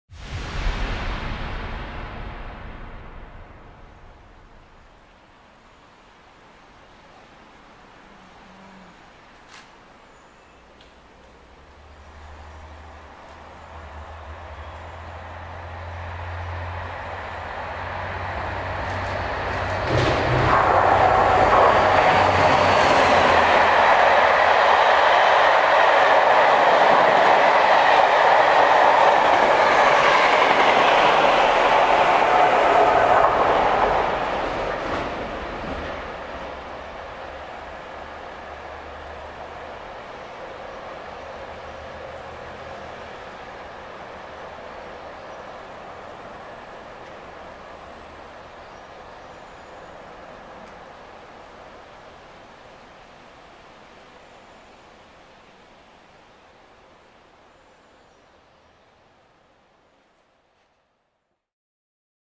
57601 & 57314 - 1Z19 Nottingham-Ely Northern Belle 26/08/2018
57601 accelerates through Melton Mowbray station leading the 1Z19 Nottingham-Ely Northern Belle, with 57314 on the back.